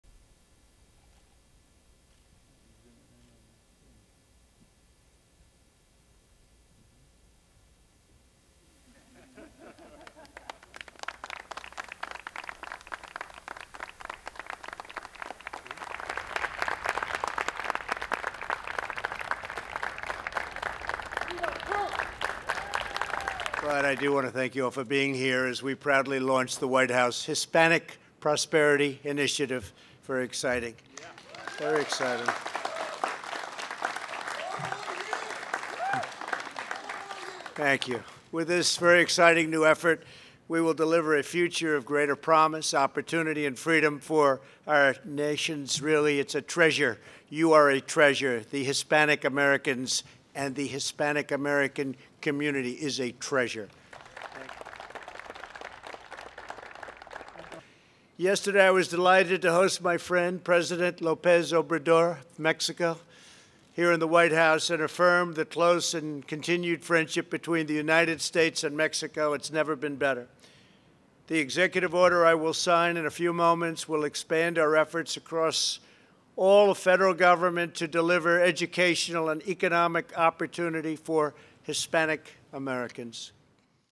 Courting Hispanic voters, Trump creates commission
President Donald Trump signs an executive order creating a new advisory commission tasked with improving Hispanic Americans' access to economic and educational opportunities, a push that comes as he hopes to increase his share of the Latino vote. (July 9)